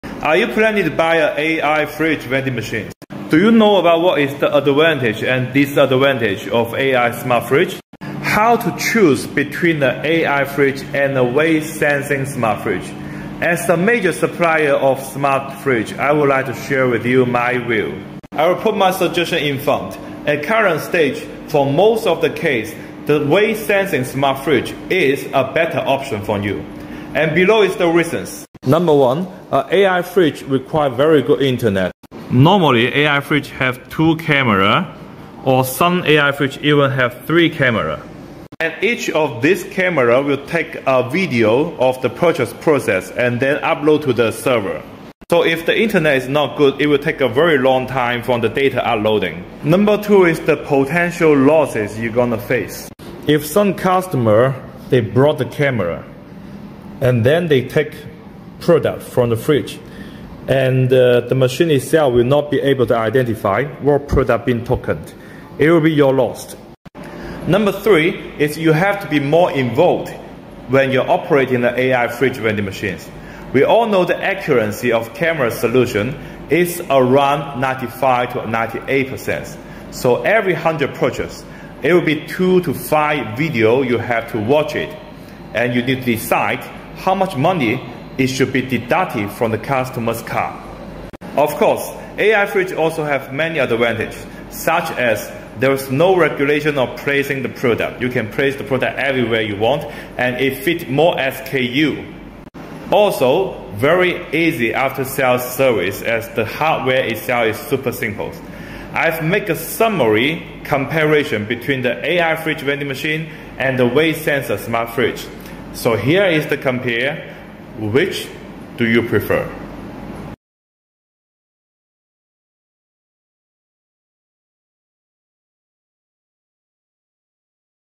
Potential problems with Camera AI smart fridge vending machine.
We received lots of inquiry of AI fridge vending machine recently, and every body is saying it's the future of unmanned retail.
Yes, I do think so.
While, there is also some potential problems with AI smart fridge vending machine~

are you planning to buy a AI fridge vending machine do you know about what is the advantage and disadvantage of AI smart fridge how to choose between the AI fridge and a weight sensing smart fridge
as the major supplier of smart fridge I would like to share with you my view I will put my suggestion in front at current stage for most of the case the weight sensing smart fridge is a better option for you and below is the reasons

No. 1 a AI fridge require very good internet  normally AI fridge have 2 cameras or some AI fridge even have 3 cameras  and each of these camera will take a video of the purchase process and then upload to the server so if the internet is not good it will take a very long time from the data uploading

No.2 is the potential losses you gonna face if some customer they block the camera and then they take product from the fridge and the machine itself will not be able to identify what product been tokened it will be your lost

NO. 3 is you have to be more involved when you're operating the AI fridge vending machines we all know the accuracy of camera solution is around 95 to 98% so every hundred purchase it will be 2 to 5 video you have to watch it and you need to decide how much money it should be deducted from the customer's card

of course AI Fridge also have many advantages  such as there's no regulation of placing the product you can place the product everywhere you want and it fit more SKU also very easy after sales service as the hardware itself is super simple.

I've made a summary comparison between the AI Fridge vending machine and the weight sensor smart fridge so here is the compare which do you prefer